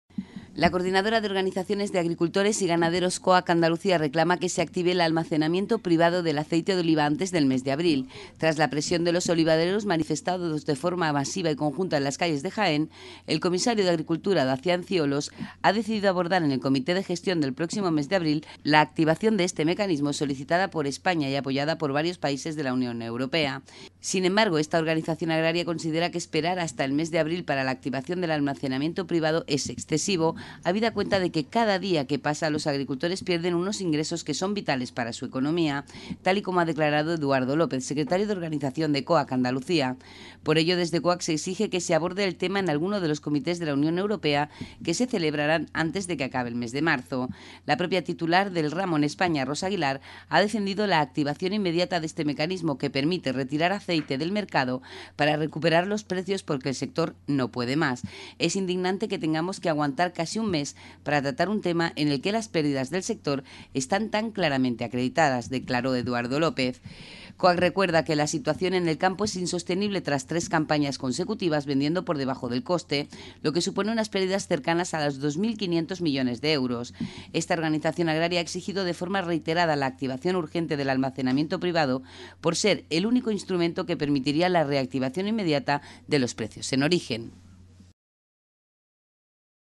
Almería Noticias Canal 28 - COAG pide el almacenamiento privado de aceite para antes de abril
La Coordinadora de Organizaciones de Agricultores y Ganaderos, COAG  Andalucía, reclama que se active el almacenamiento privado del aceite de oliva antes del mes de abril. Tras la presión de los olivareros manifestado de forma masiva y conjunta en las calles de Jaén, el Comisario de Agricultura, Dacian Ciolos, ha decidido abordar en el Comité de Gestión del próximo mes de abril la activación de este mecanismo, solicitada por España y apoyada por varios países de la UE.